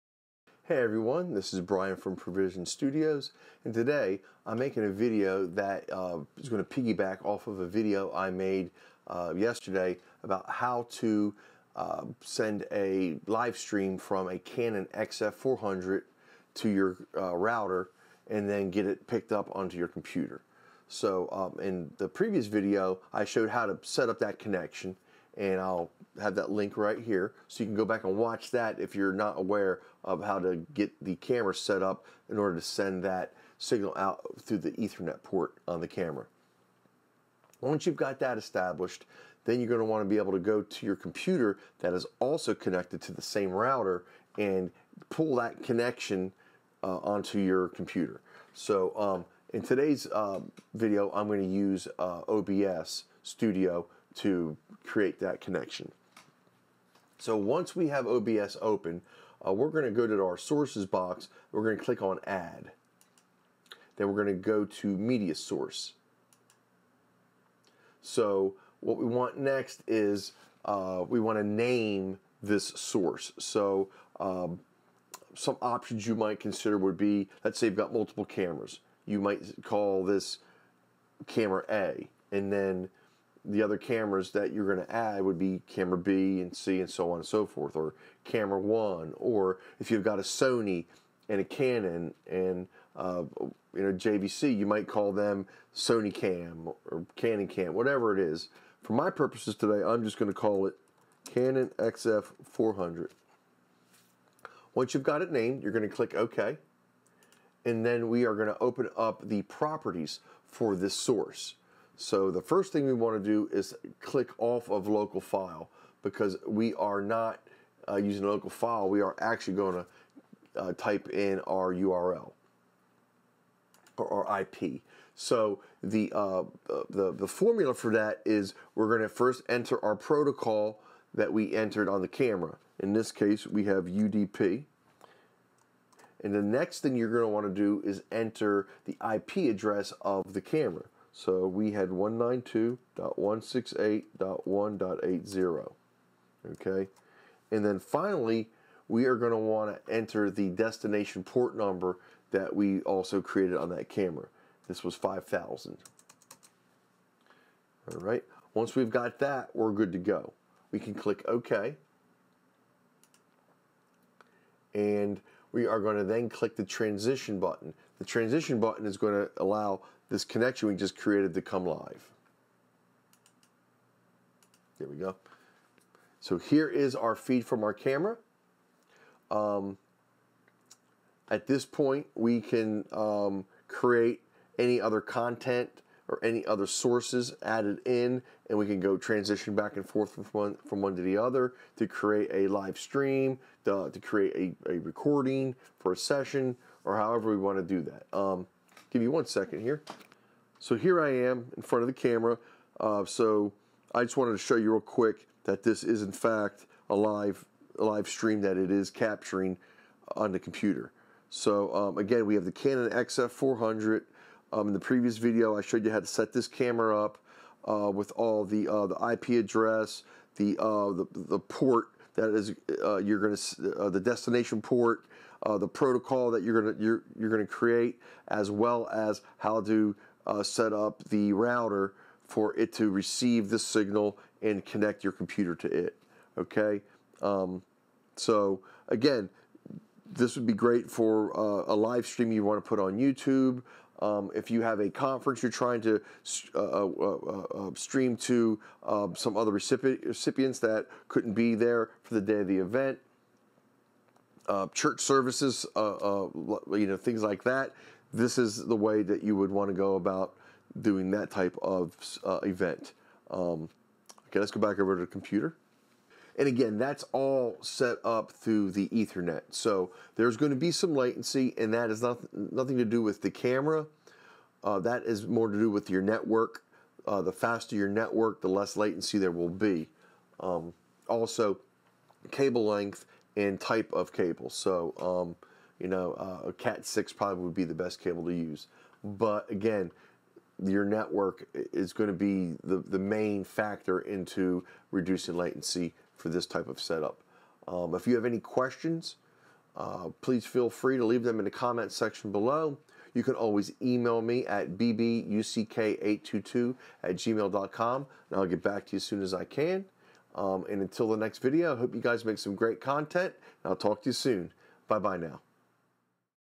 PART 2: Canon XF400 • Live Streaming Over IP
In this video, Brian shows us how to set up OBS Studio to receive the live stream feed off of the Canon XF400 camcorder. This is part 2 of a two-part video.